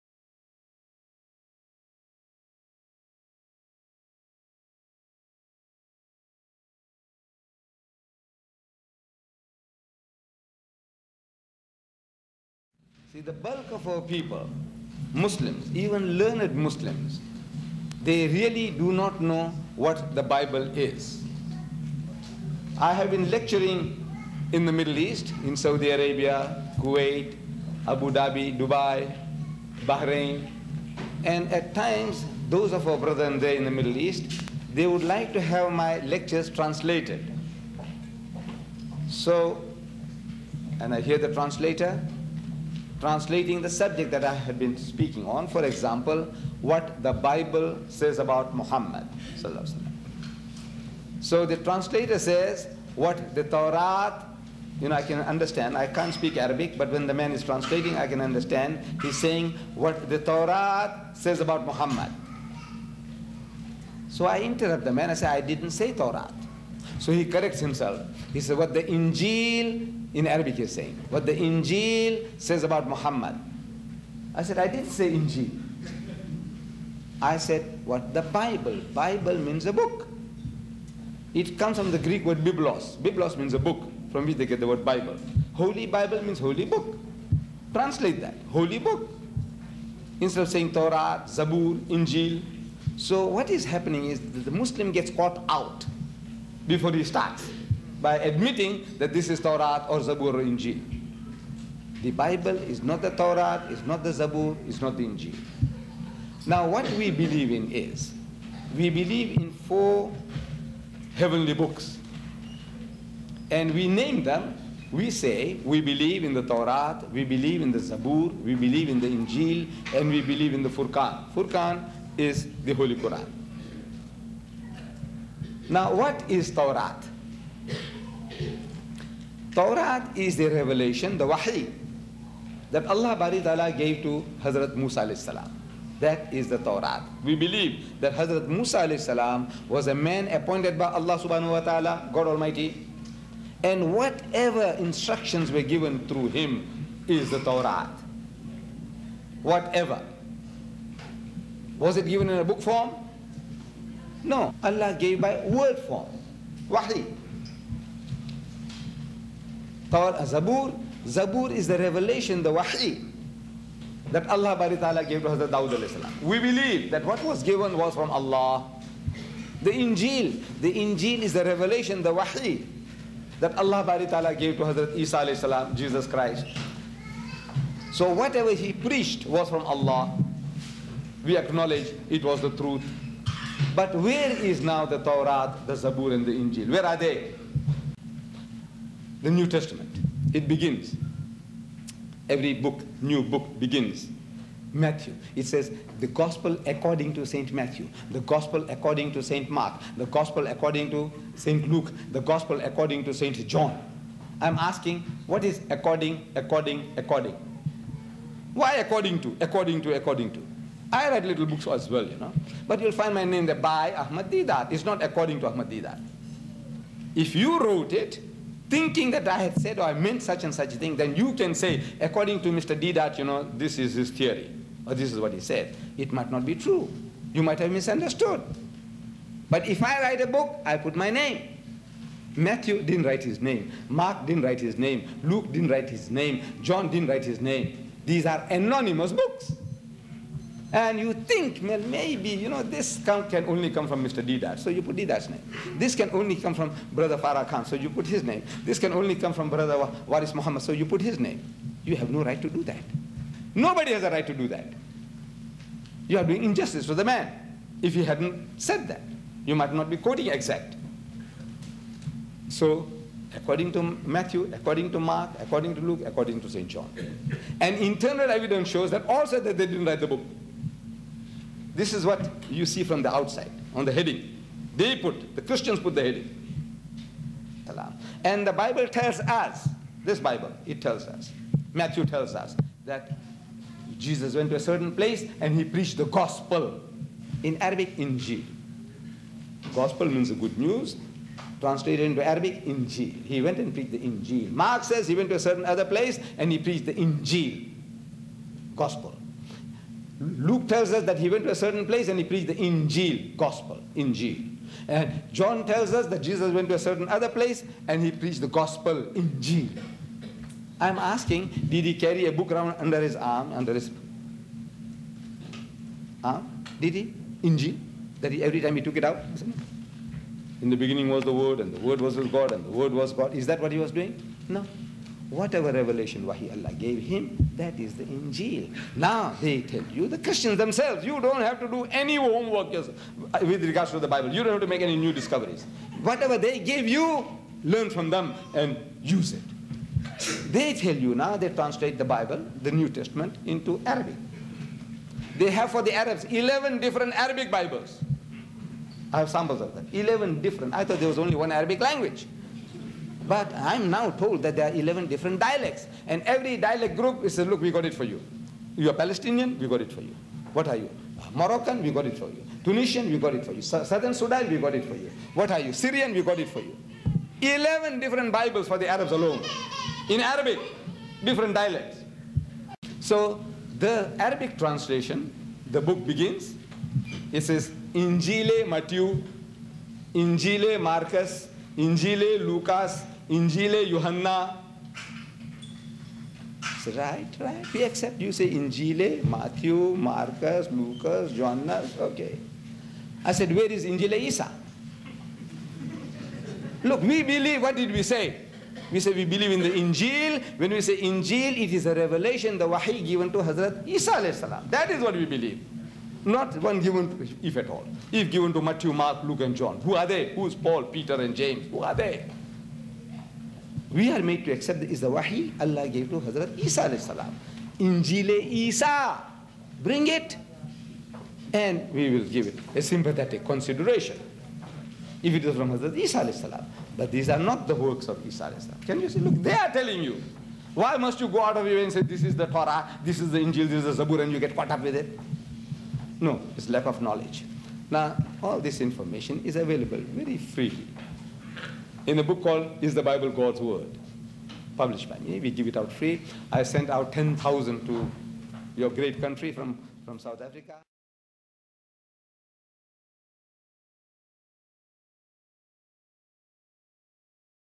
Difference between Bible and Injeel إنجيل.
Some Muslims think that the Bible is the Gospel إنجيل mentioned in Quran but the Gospel إنجيل in Quran is the Gospel given or taught by Jesus but the bibles don't have any book named Gospel of Jesus, but in contrary it has Gospel by Mark, Luke, John, Matthew.